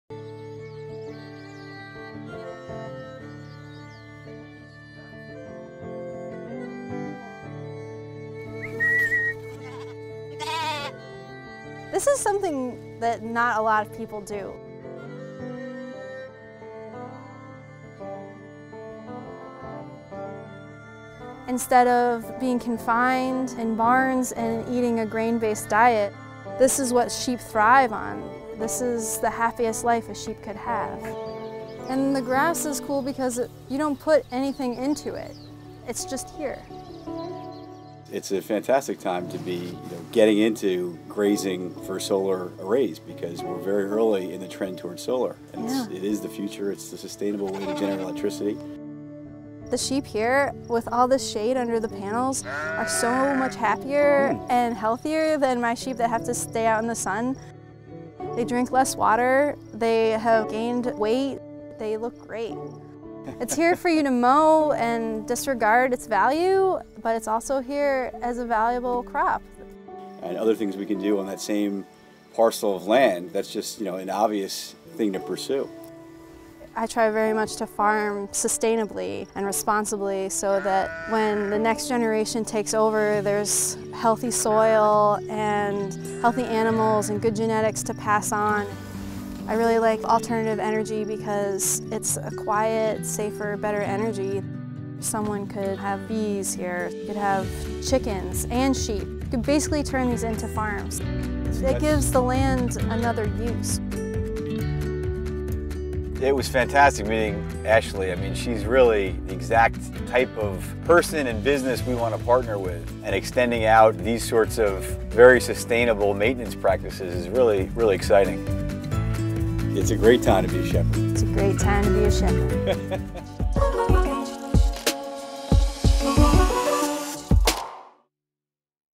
Solar and...Sheep? Solar Grazing Produces Clean Energy While Enriching Wildlife
PowerFlex (formerly EnterSolar) deployed its first solar grazing project at a 2.4-MW ground-mounted installation in Halfmoon, New York. The sheep feed on the vegetation around the ground-mounted solar panels, which helps keep them free of debris and functioning properly. See how PowerFlex can unlock the clean power potential of your sites